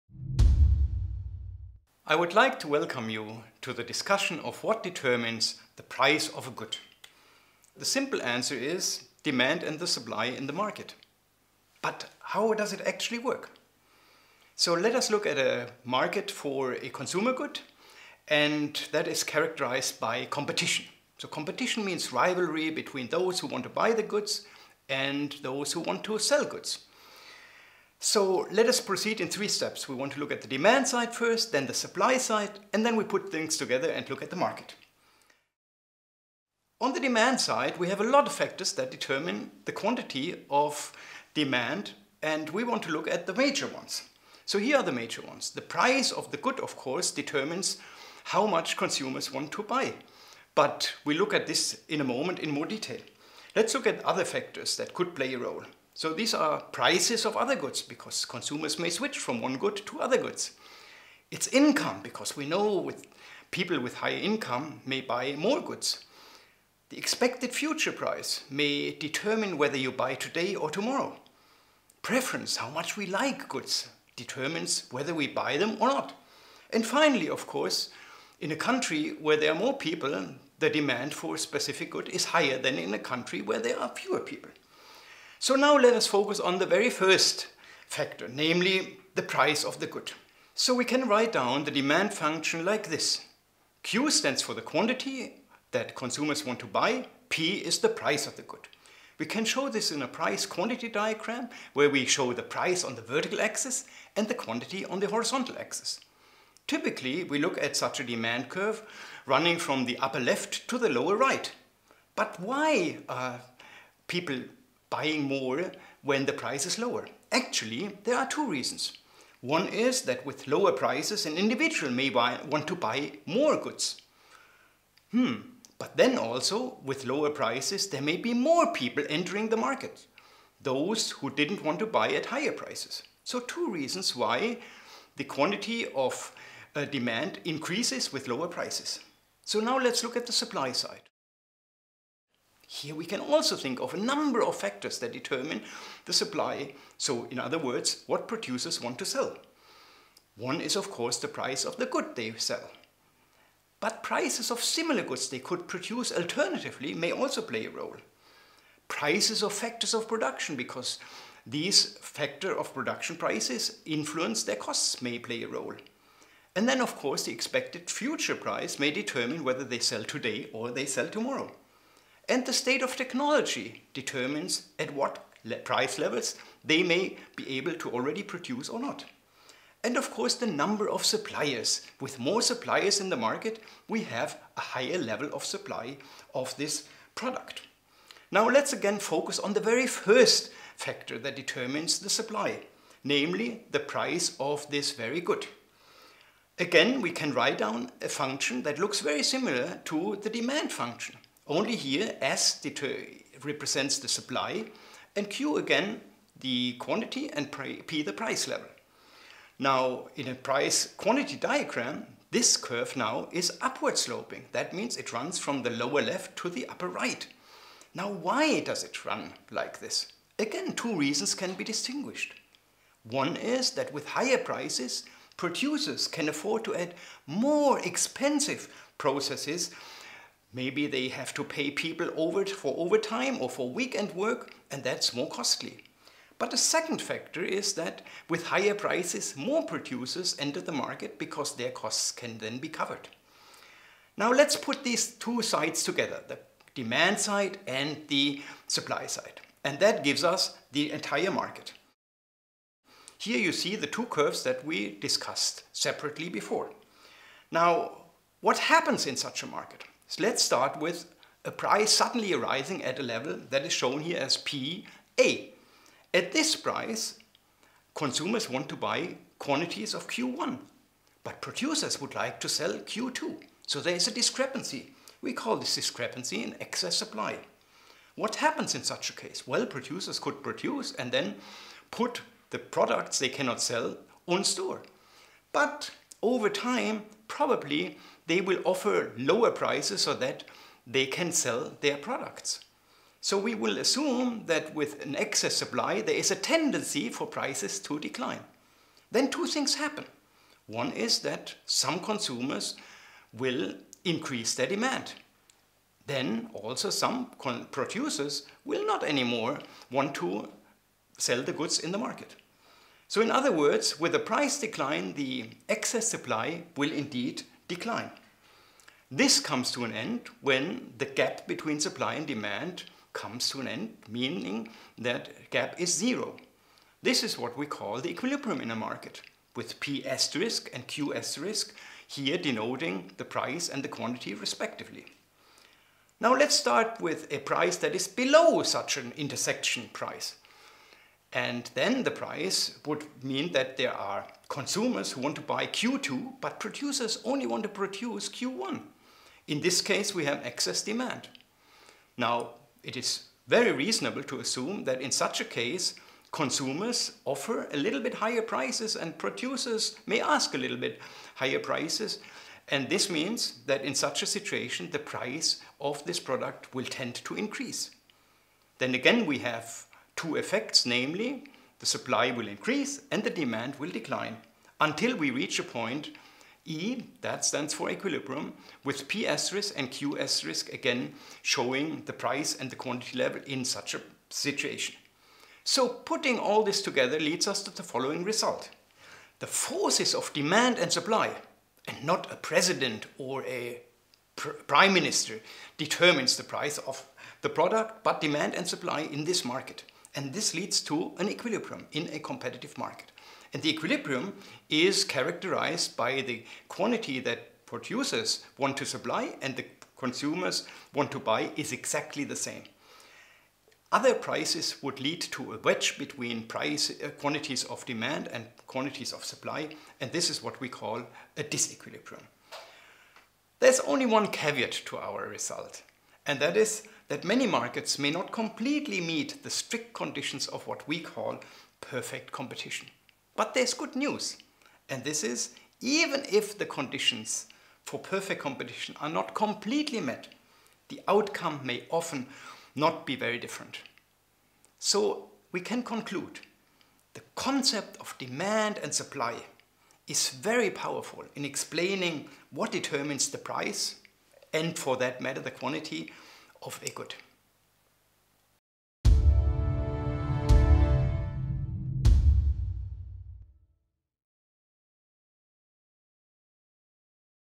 The Interaction of Supply and Demand | Tutorial by WHU Professor Dr. Michael Frenkel
What determines the price of a specific good? Learn how supply and demand interact in a competitive market. This video is easy to follow and does not require any prior knowledge of business or economics. The target group of the video are students of economics and business at the beginning of their studies of markets and price determination.

Professor Dr. Michael Frenkel holds the Chair of Macroeconomics and International Economics at WHU – Otto Beisheim School of Management. His research focuses on international finance, international financial markets, open economy macroeconomics, monetary policy, and European integration.